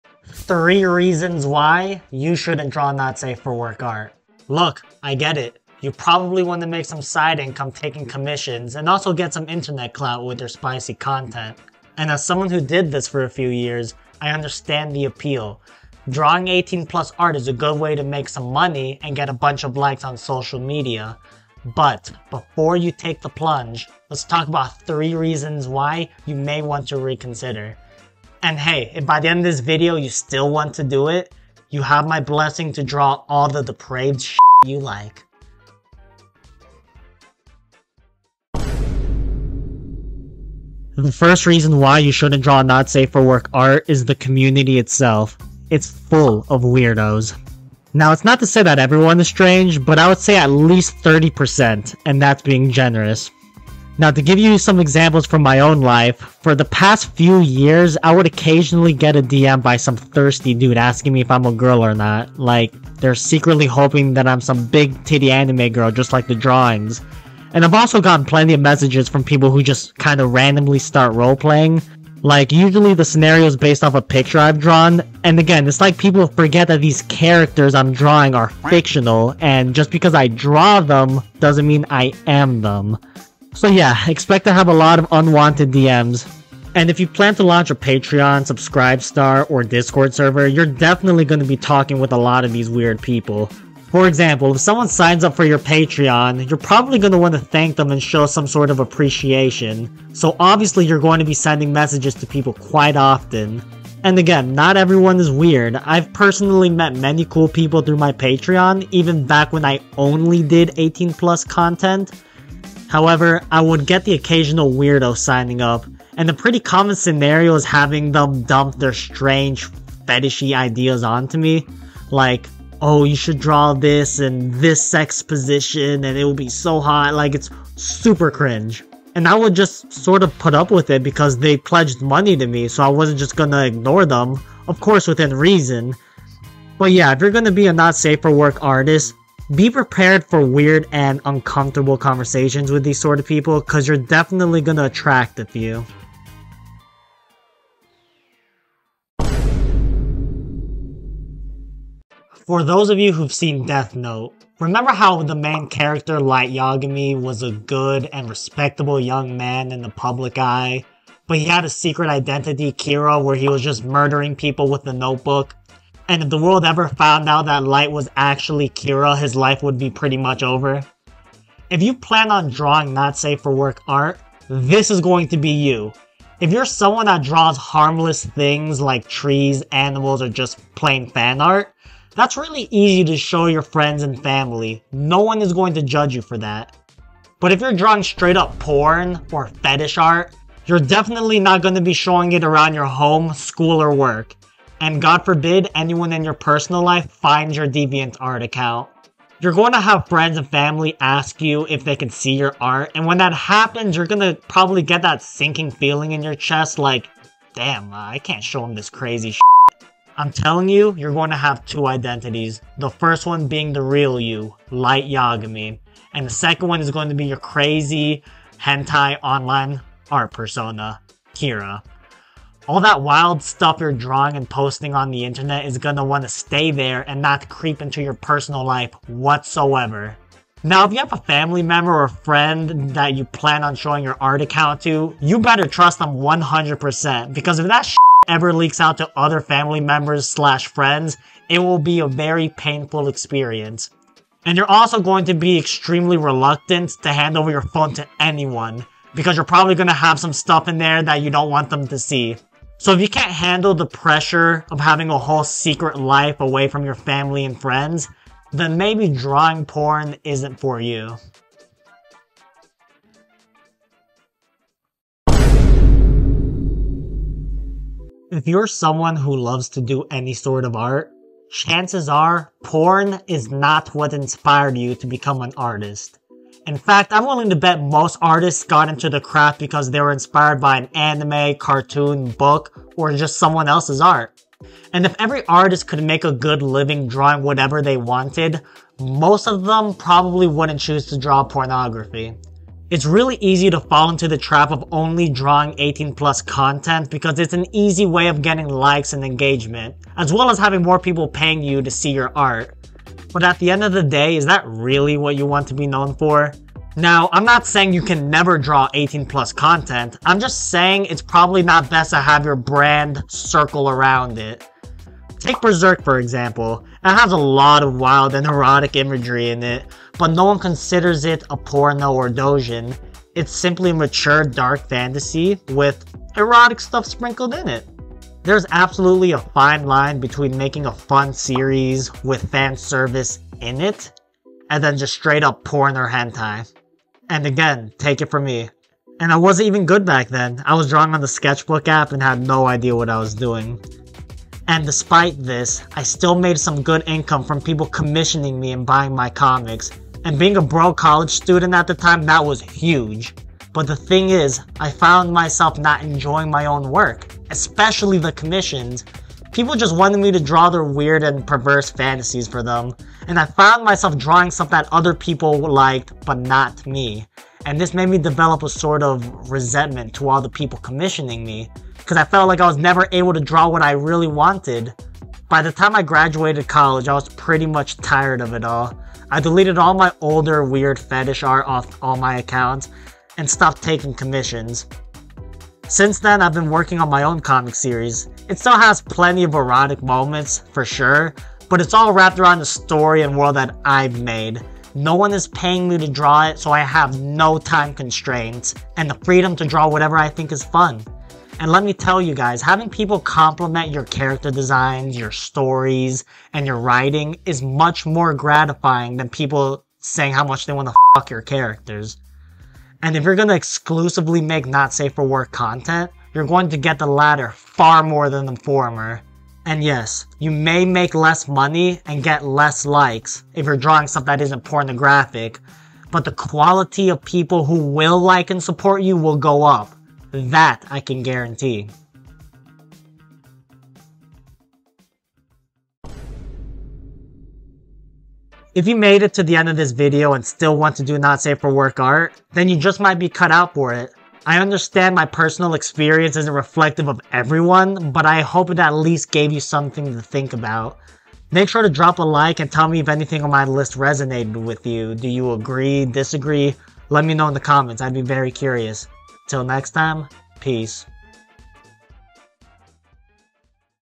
3 Reasons NOT To Draw NSFW Art
Wanna discuss your art ideas with me?

🔍  Time Stamps 🔍
0:00 Intro
0:37 Weirdos
2:28 Two Lives
4:24 Life Purpose
7:15 Conclusion

🎨 Unlocking the NSFW Art World: 3 Reasons to Think Twice Before Taking the Plunge

So, you're thinking about diving into the world of NSFW art, drawn in by dreams of creative expression, commissions, and perhaps a touch of the unconventional. I totally get it. As someone who explored this realm for a few years, I've got insights to share.

In this video, we'll delve into three compelling reasons why becoming a NSFW artist might not be the smooth artistic journey you envision. From the quirky dynamics of the NSFW community to the constant juggling act of living a double life, we'll cover it all.

🔍 Video Highlights:

The Weird NSFW Community: Uncover the unique dynamics of the NSFW art community and why lots of creeps might be asking if you're female. 🕵️‍♂️
Awkwardness in Personal Life: Explore the challenges of living a double life, keeping your NSFW art hidden from friends and family. 🤷‍♀️
Reputation and Artistic Identity: Consider the long-term impact of being known solely for explicit content and whether it aligns with your broader artistic goals. 🎨
🤔 Still Curious?
If, by the end of this video, you still want to take the plunge into the darkness of NSFW art, you have my blessing. Art is a personal journey, after all.